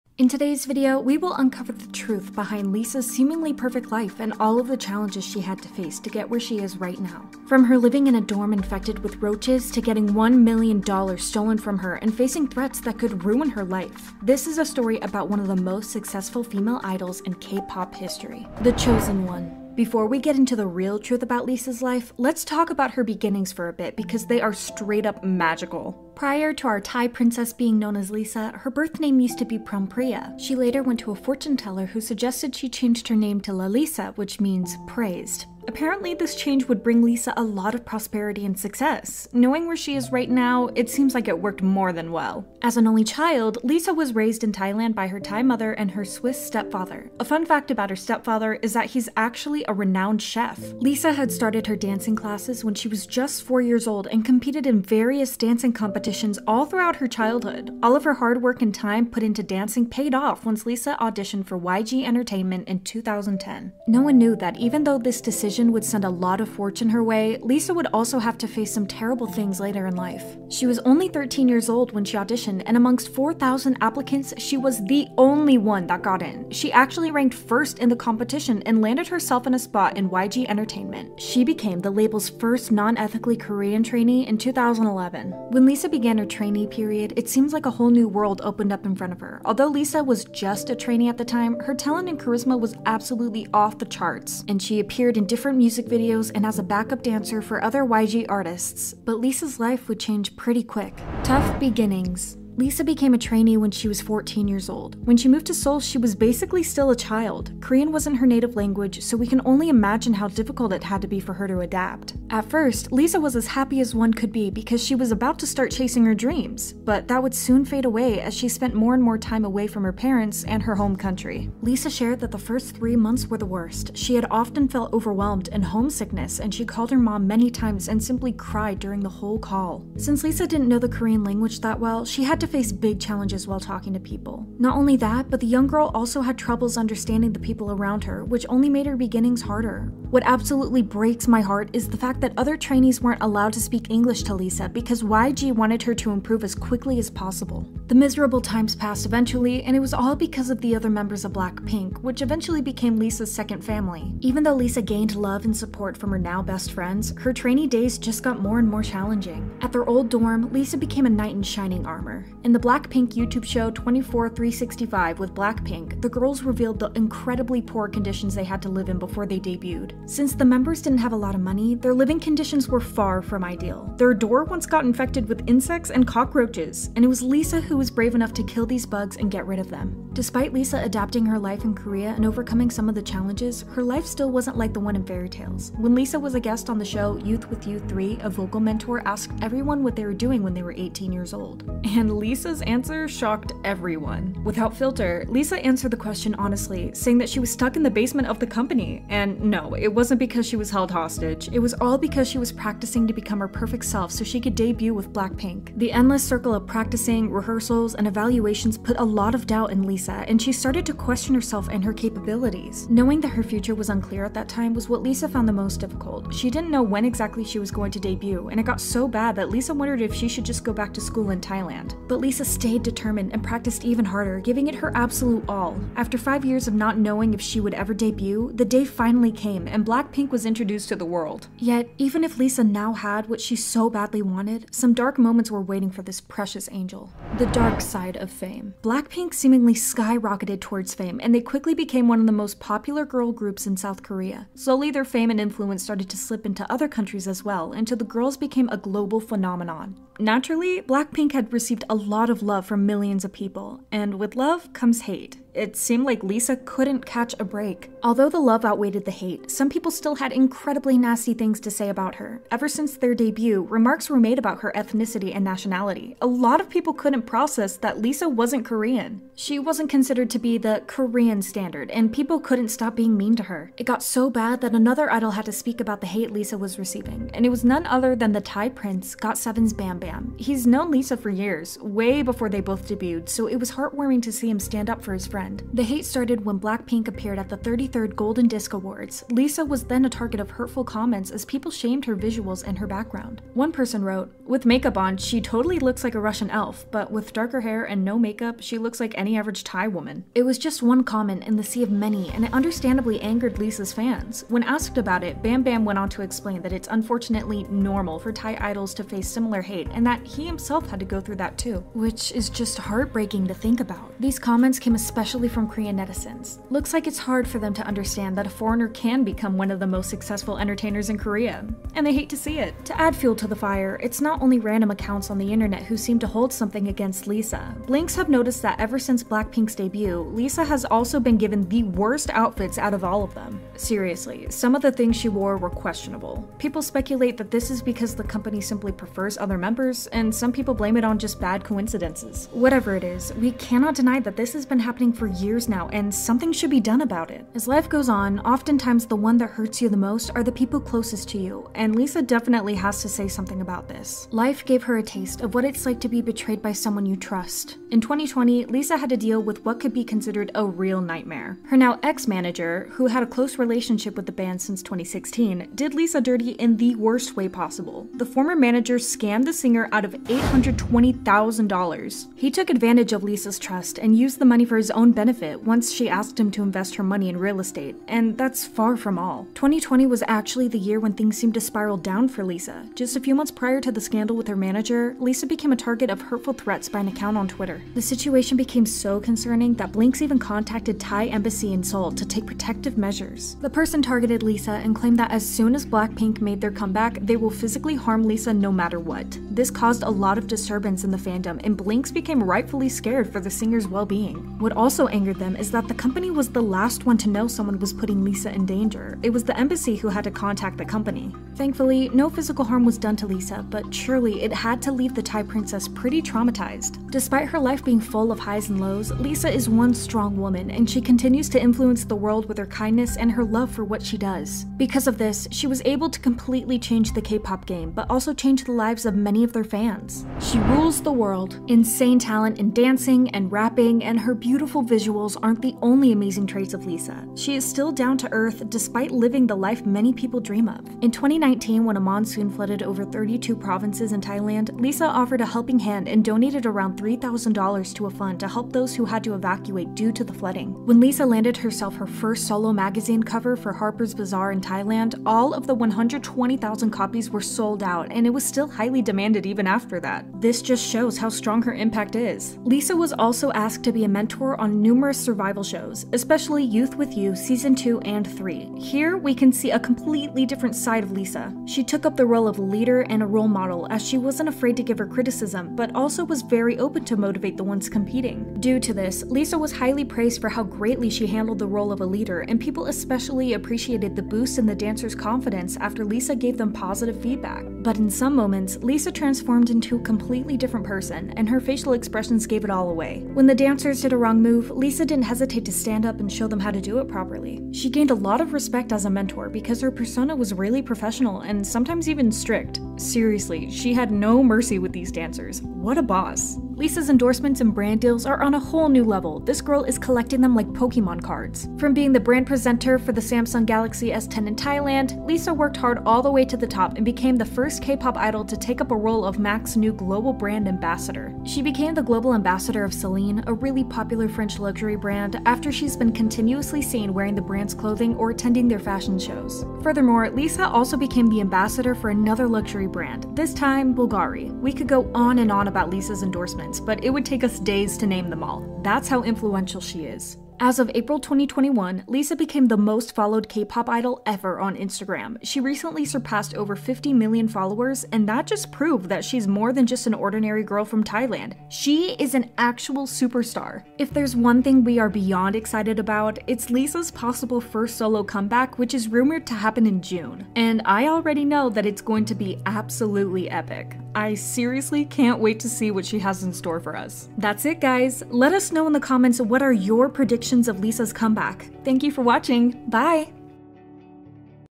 What Nobody Understands About Blackpink's Lisa
blackpink's lisa has quickly become one of the most successful kpop idols. but what's the truth behind her career and where is she heading now? from living in a dirty dorm, getting $1 million stolen from her to becoming the leading star of our generation. this is lisa from blackpink...